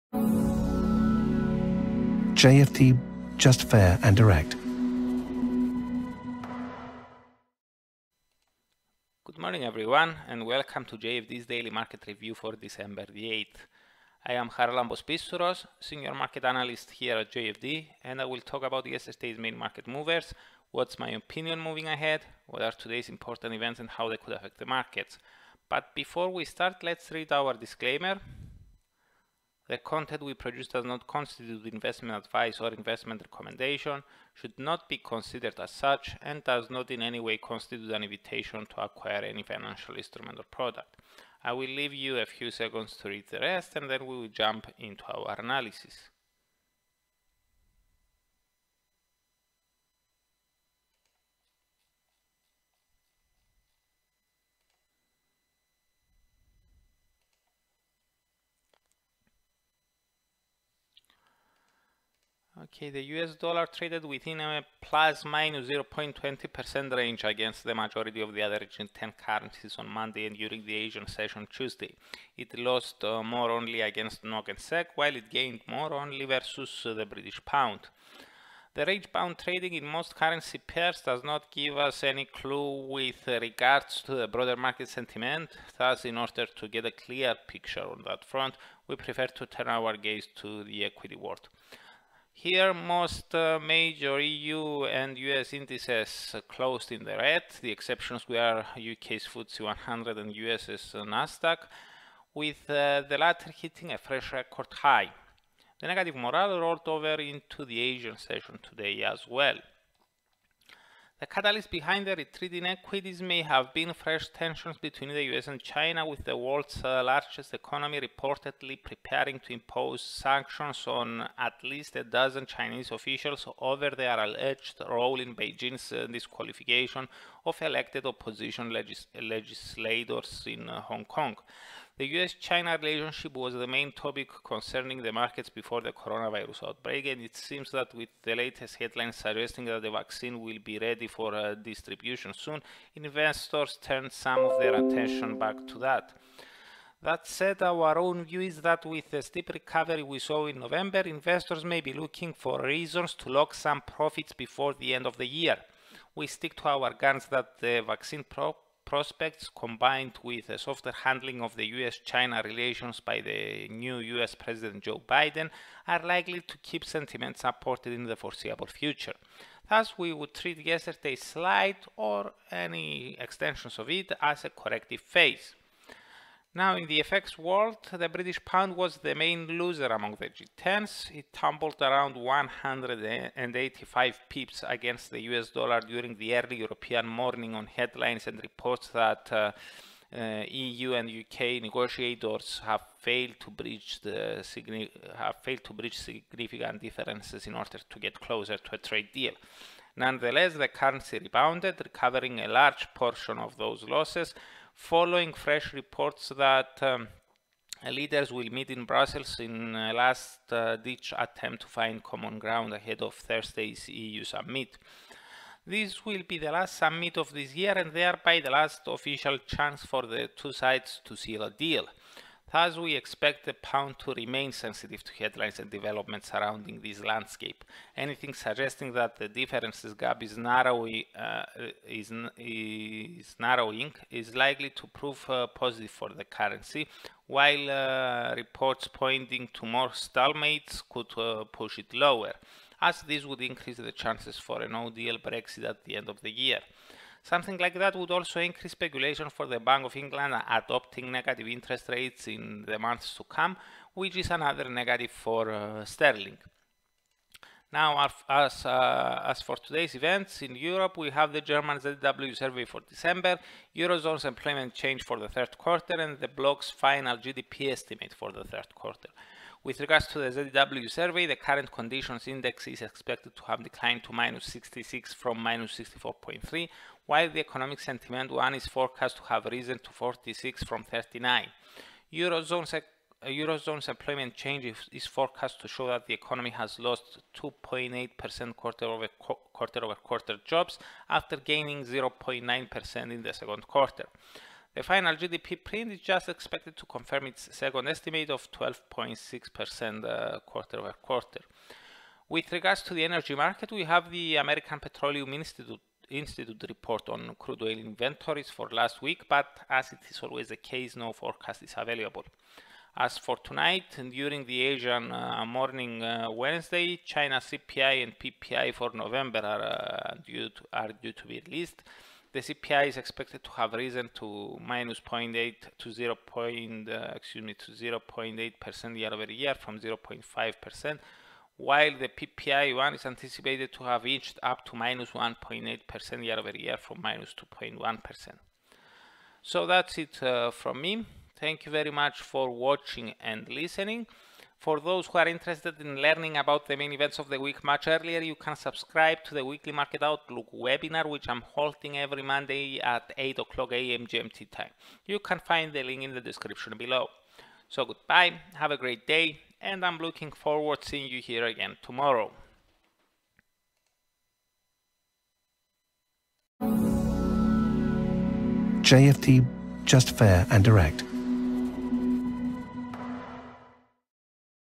Daily Market Review: Equities Retreat, Brexit Headlines Drive the Pound
Most major global equity indices traded in the red yesterday and today in Asia, with the catalyst perhaps being fresh tensions between the US and China. In the currency world, the pound was the main loser among the G10s. It initially fell off the cliff on reports that the EU and the UK were still unable to find consensus over trade, but it rebounded thereafter, following reports that leaders will meet in Brussels in a last-ditch attempt to solve their differences.